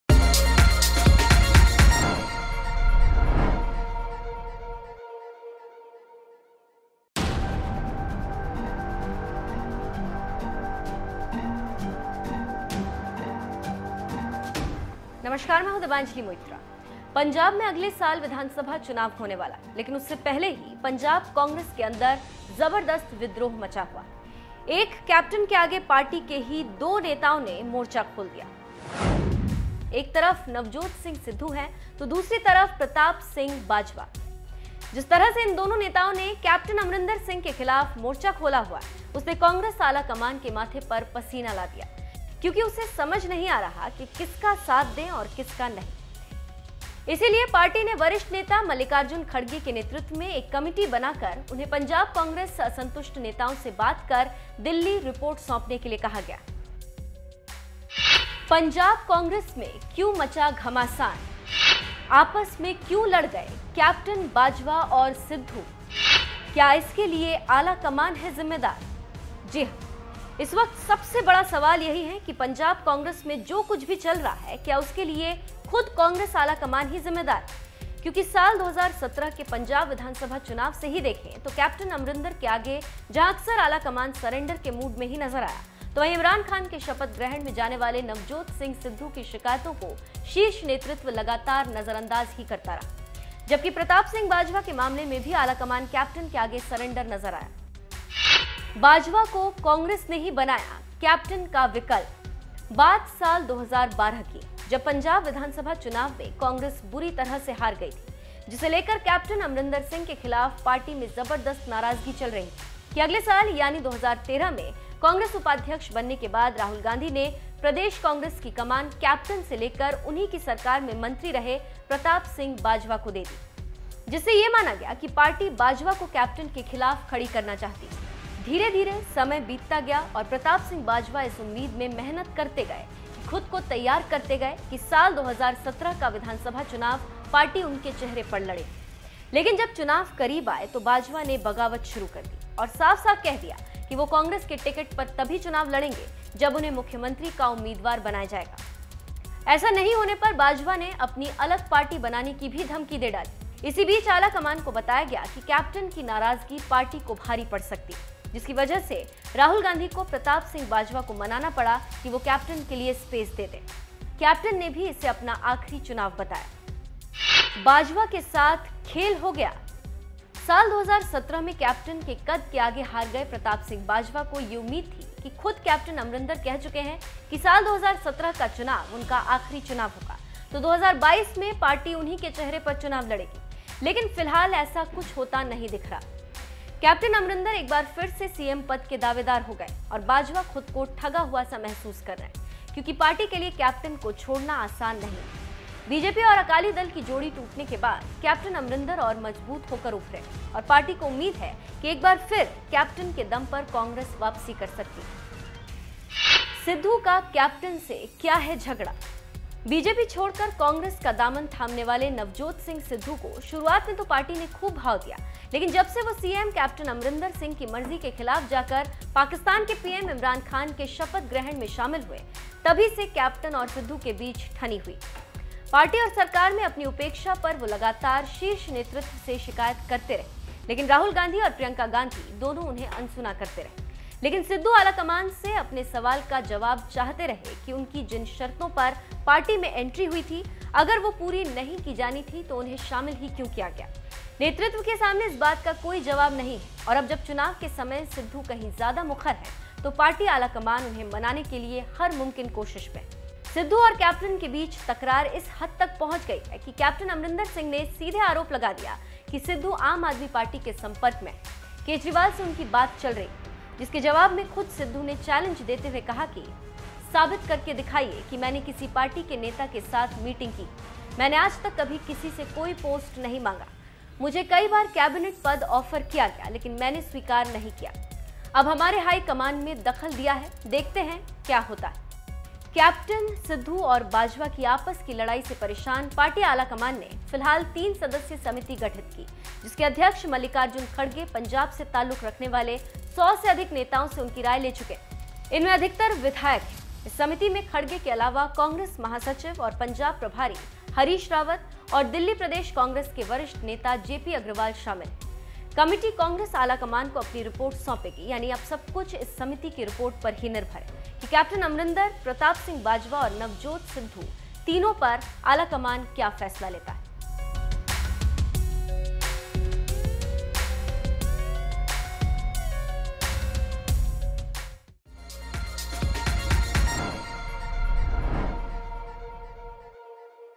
CM Amrinder से भिड़े सिद्धू और बाजवा तो कांग्रेस आलाकमान के छूटे पसीने!
एक तरफ नवजोत सिंह सिद्धू हैं तो दूसरी तरफ प्रताप सिंह बाजवा हैं... जिस तरह से इन दोनों नेताओं ने कैप्टन अमरिंदर सिंह के खिलाफ मोर्चा खोला हुआ है... उसने कांग्रेस आलाकमान के माथे पर पसीना ला दिया है !